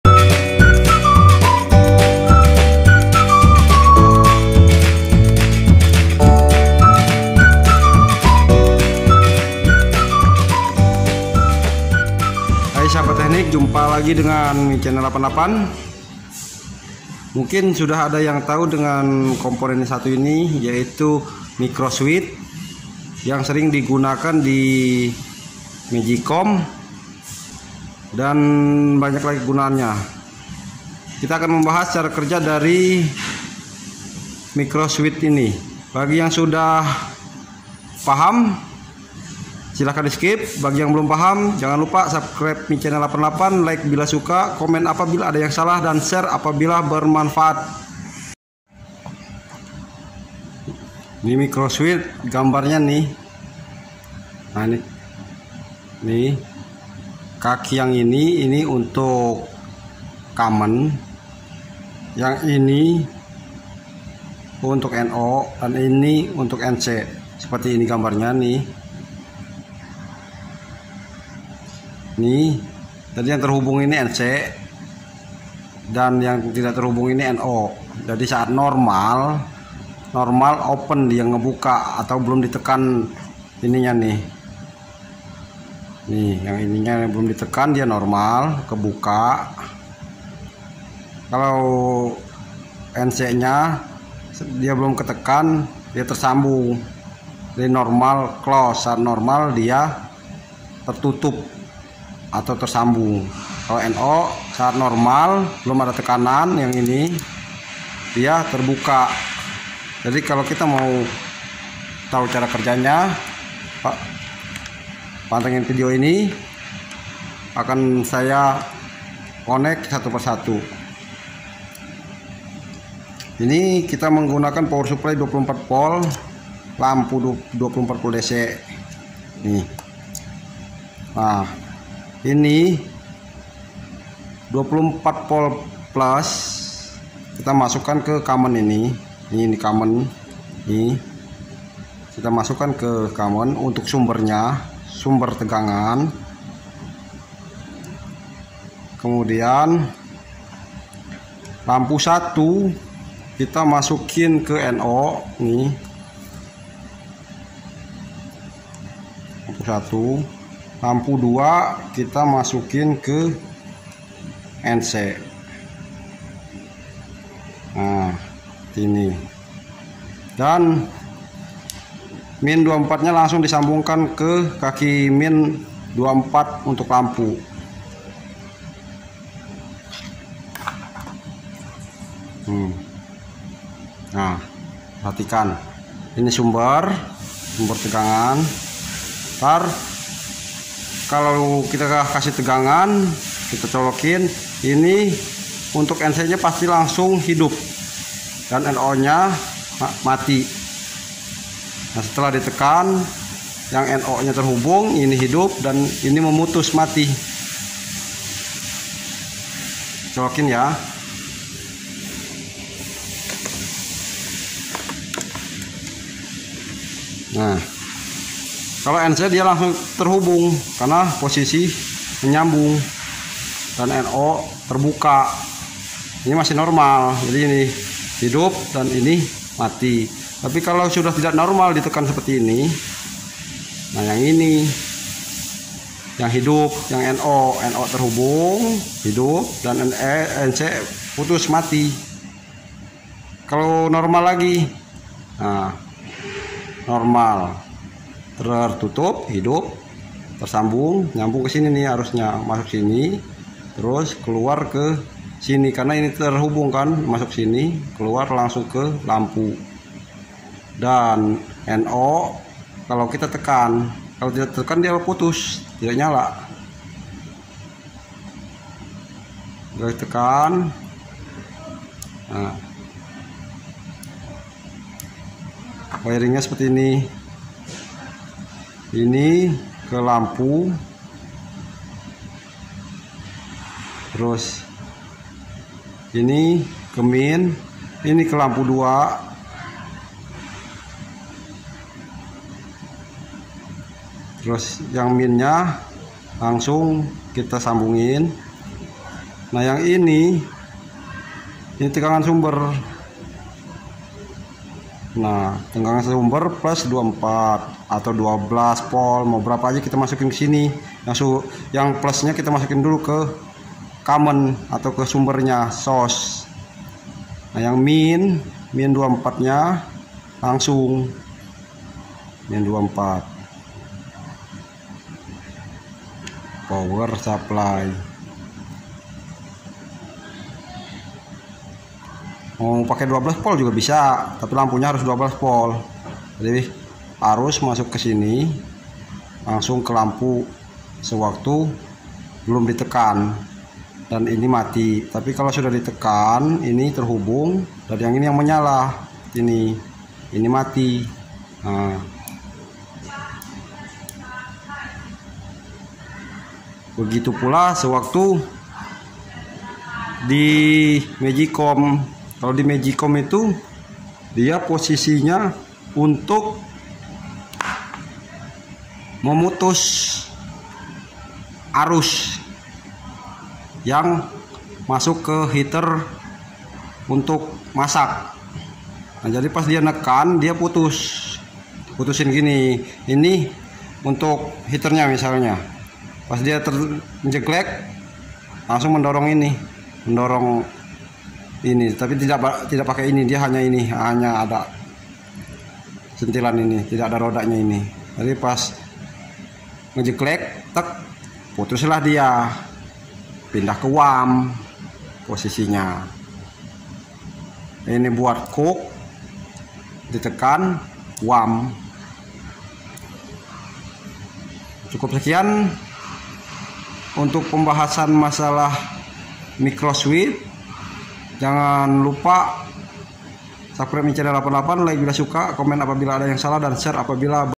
Cara Kerja Micro Switch | Micro Switch Working | Micro Limit Switch Connection
Micro switch sering digunakan diperalatan rumah tangga,contohnya Magic com,disini akan dijelaskan cara kerjanya dan penyambungan kabelnya kaki mana aja yg digunakan,simak baik2 bagi yg belum paham,bagi yg sudah paham,silahkan skip video ini,selamat menyaksikan

Micro switches are often used in household appliances, for example Magic com, here we will explain how they work and which cable connections are used, listen carefully for those who don't understand, for those who already understand, please skip this video, enjoy watching